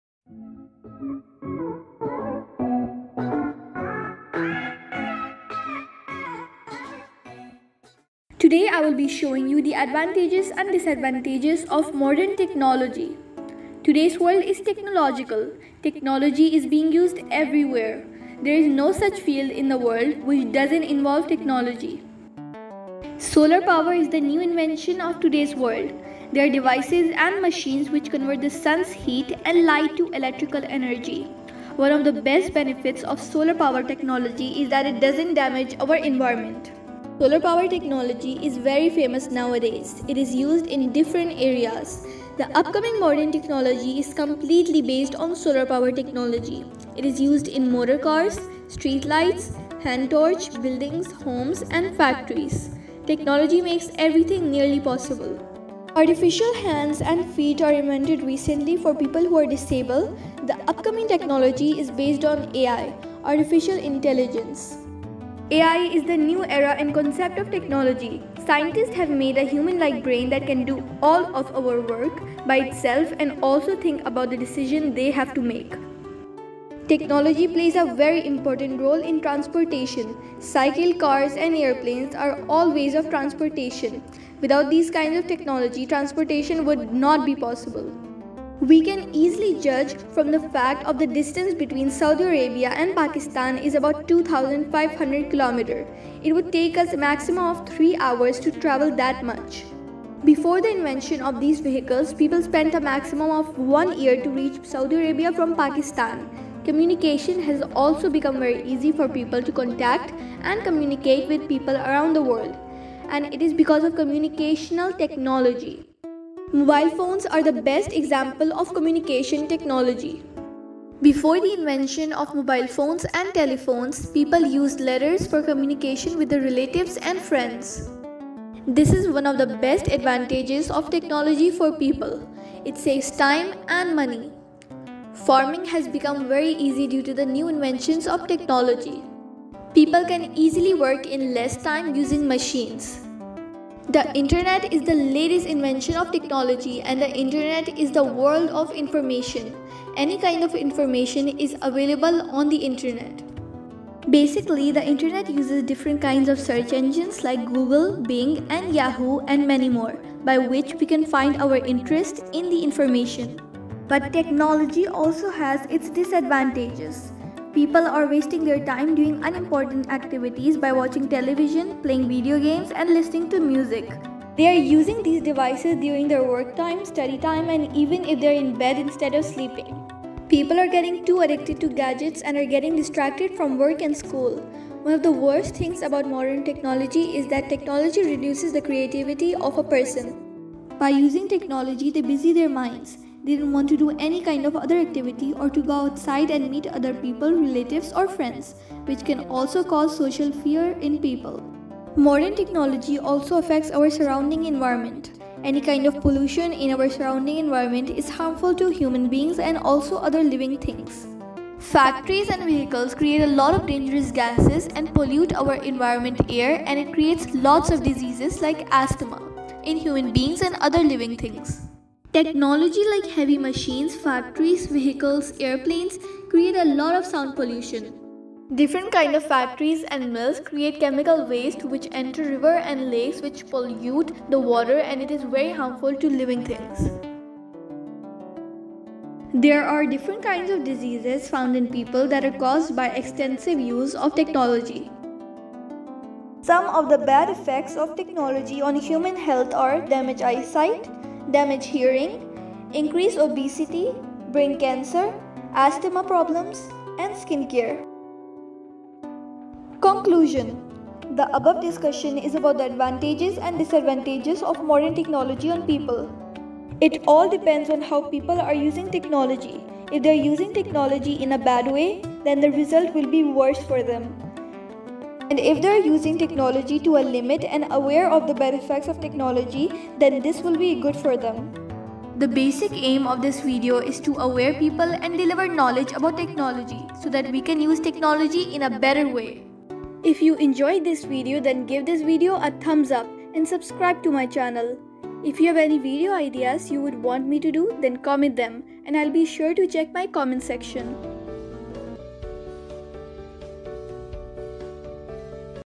Advantages and disadvantages of technology - Alizay Khan
Nowadays, technology is being used everywhere

technology might be beneficial but dont forget it has its bad effects.